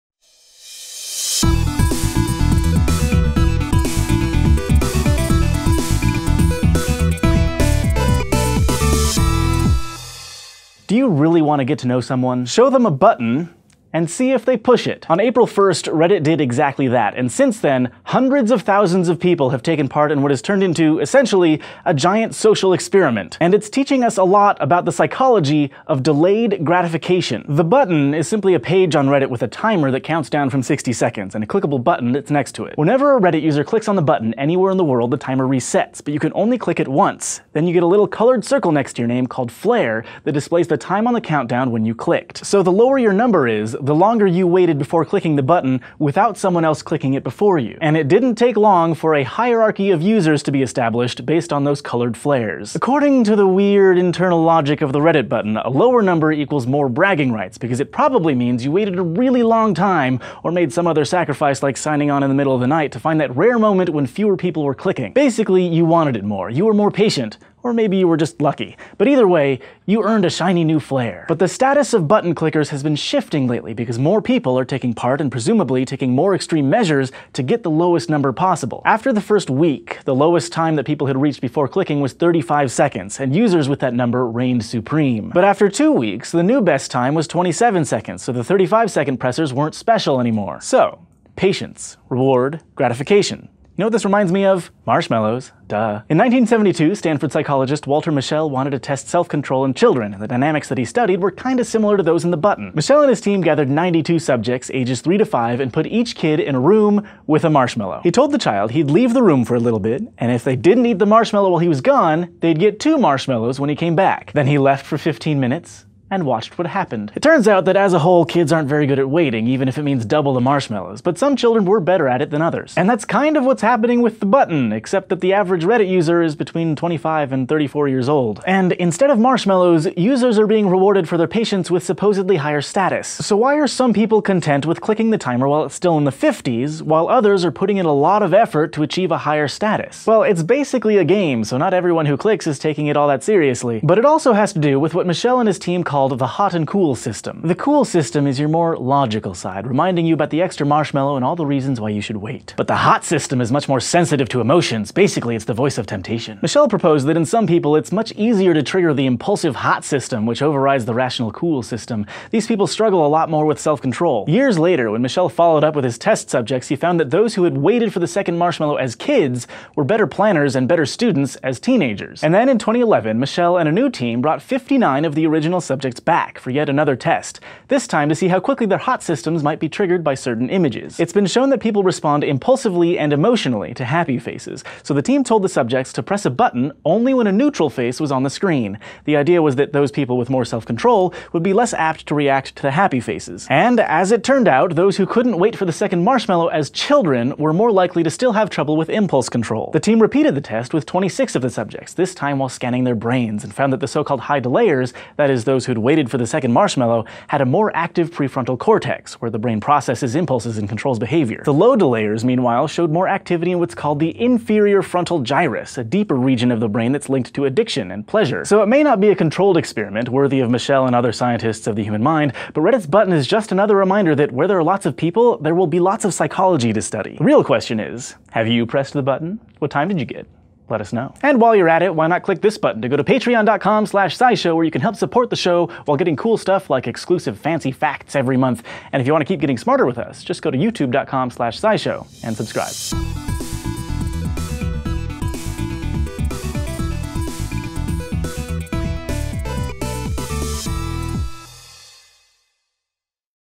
The Psychology of The Button
Hundreds of thousands of people have clicked a button on reddit. Turns out that when you click can reveal a lot about your brain, and human nature.